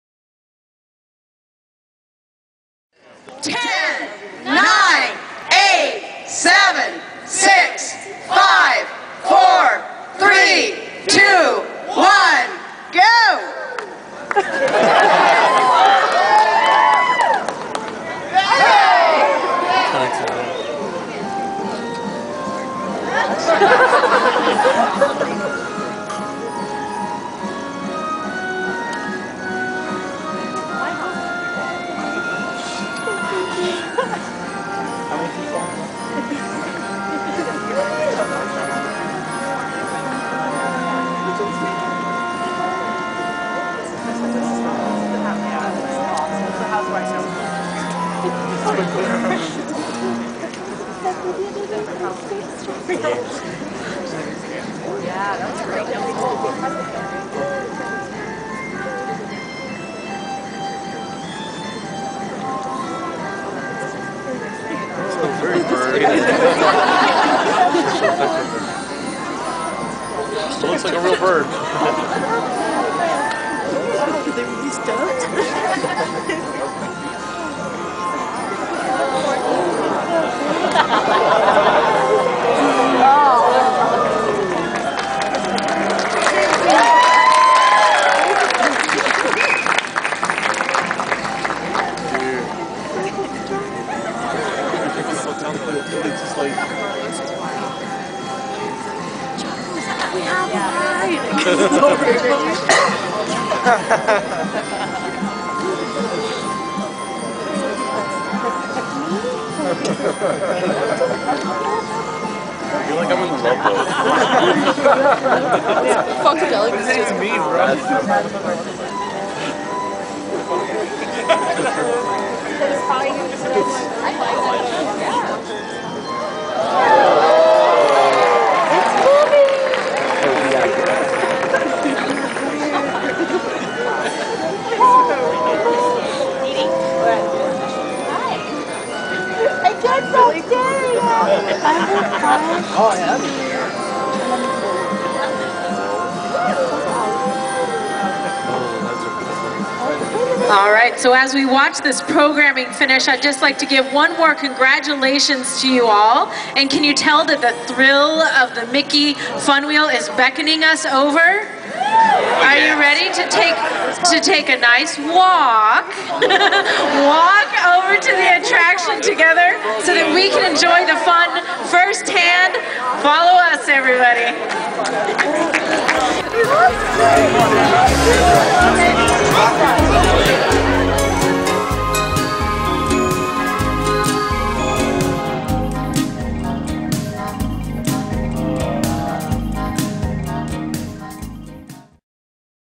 Mickey's Fun Wheel Cast Member Lighting Ceremony
On May 12, 2009 Cast Members were invited to watch the dedication lighting ceremony of Mickey's Fun Wheel. Guest speakers included the Disneyland Resort Ambassadors, Disneyland President Ed Grier, Disney's California Adventure Vice President Mary Niven and Director / Sr. Show Producer for WDI Lisa Girolami.

The event was scheduled to take place at 7:45pm,but they tried to postpone the actual lighting ceremony as long as they could since it was still light out. But because the event was for a short time, they tried to hold out as long as they could.